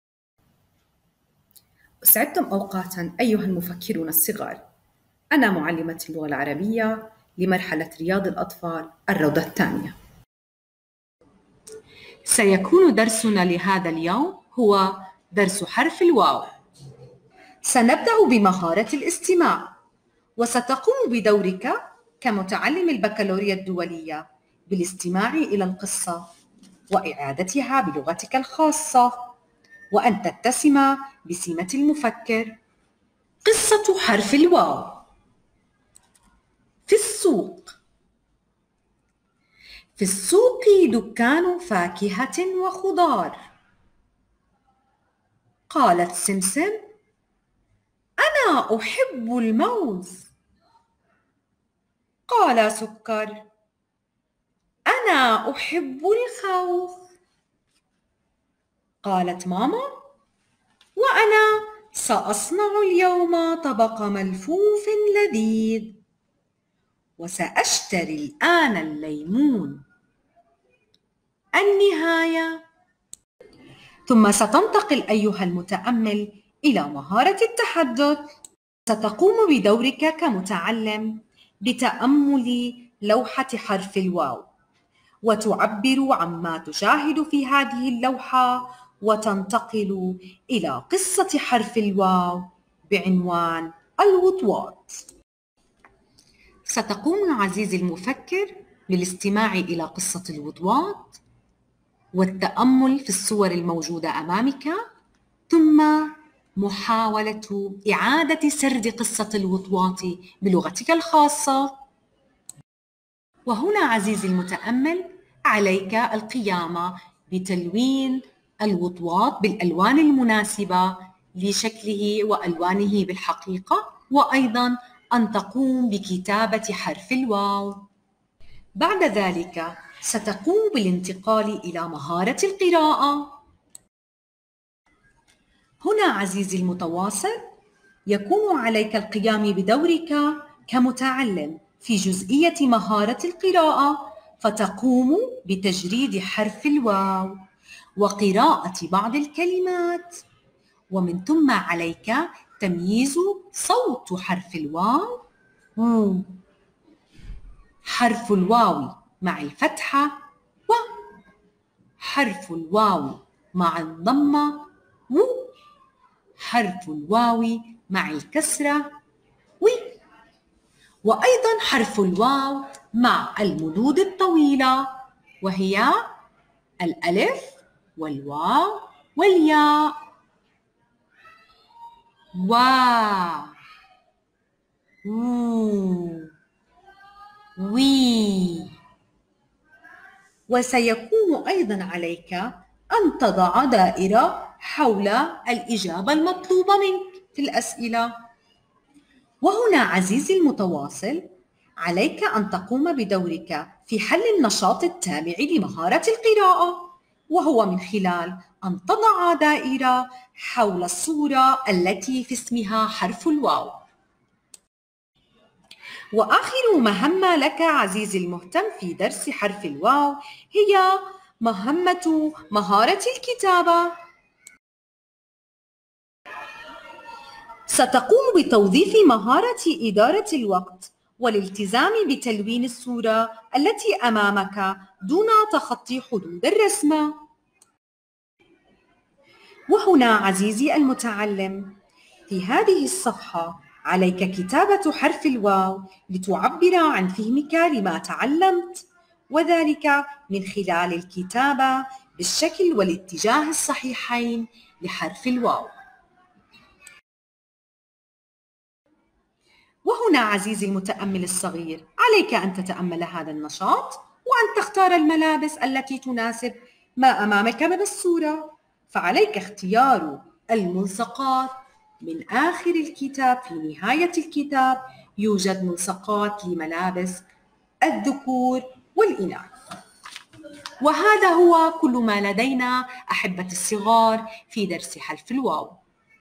حرف الواو - اللغة العربية - روضه 2 - المعلمة رويدة مشربش
حرف الواو

فيديو يتحدث عن حرف الواو   لفظ الحرف و قراءة كلمات عن الحرف في أول و وسط و آخر الكلمة و كتابة الحرف و قراءة قصة عن الحرف